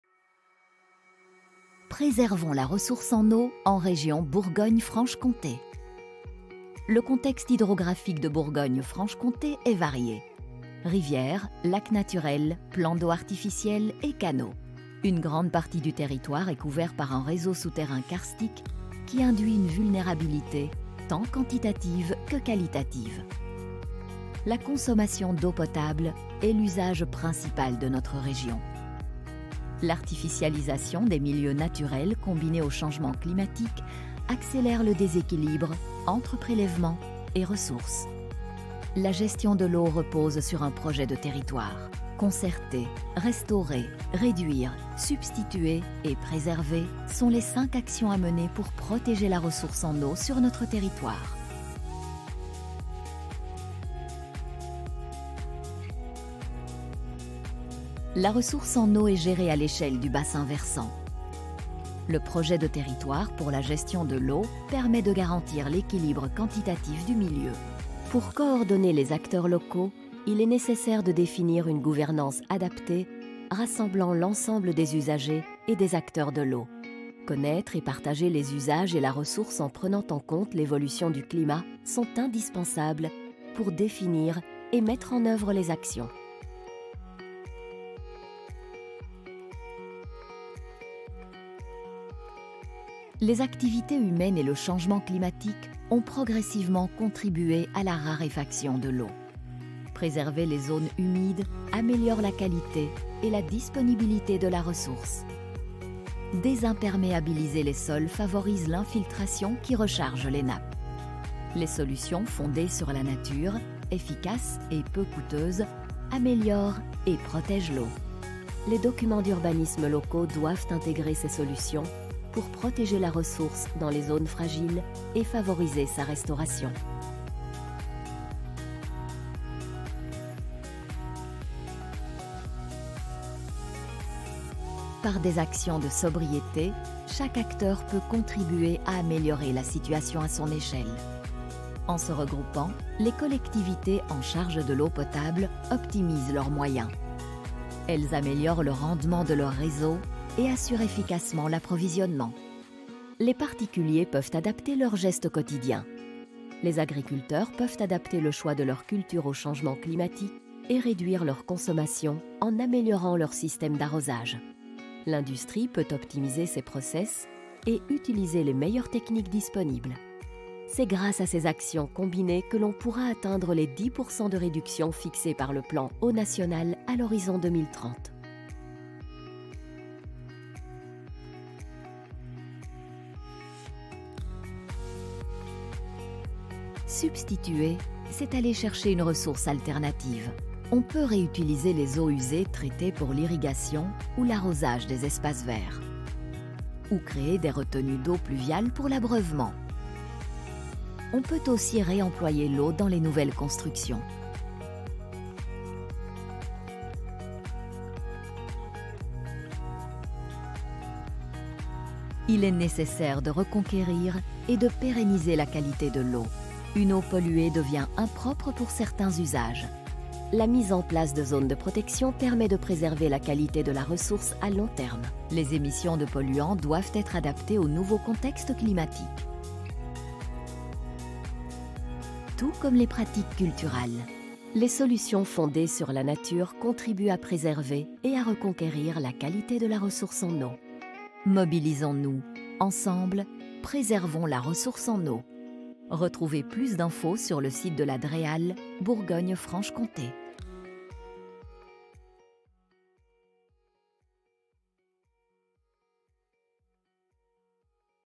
La stratégie de gestion de l'eau par la DREAL en Bourgogne-Franche-Comté
Les sécheresses à répétition que la région Bourgogne-Franche-Comté, tout comme notre pays, a connues ces dernières années et l’accélération des phénomènes climatiques extrêmes nous imposent d’aborder la gestion quantitative de la ressource en eau sous un autre angle que celui de la gestion de crise. Anticiper et limiter les déficits structurels par des actions de préservation de la ressource en eau est devenu indispensable pour préserver les milieux naturels et sécuriser tous les usages à chaque période de l’année. La récurrence des évènements exceptionnels, désormais fréquents, montre qu’il y a urgence à agir.

Ainsi, les services de l’État se mobilisent aux côtés des acteurs du territoire pour mettre en place 5 actions clés pour une gestion durable de la ressource en eau en Bourgogne-Franche-Comté :
1) Déployer des Projets de Territoire pour la Gestion de l’Eau ;
2) Mobiliser les Solutions fondées sur la Nature ;
3) Économiser l’eau et rendre les usages plus sobres ;
4) Mobiliser les ressources alternatives ;
5) Protéger la qualité des ressources en eau.

Pour plus d'informations sur cette stratégie régionale de l’État sur la gestion quantitative de la ressource en eau, rendez-vous sur le site Internet de la DREAL Bourgogne-Franche-Comté :
Stratégie régionale de l'État - 5 actions clés pour une gestion durable de la ressource en eau | Portail internet DREAL Bourgogne-Franche-Comté (developpement-durable.gouv.fr)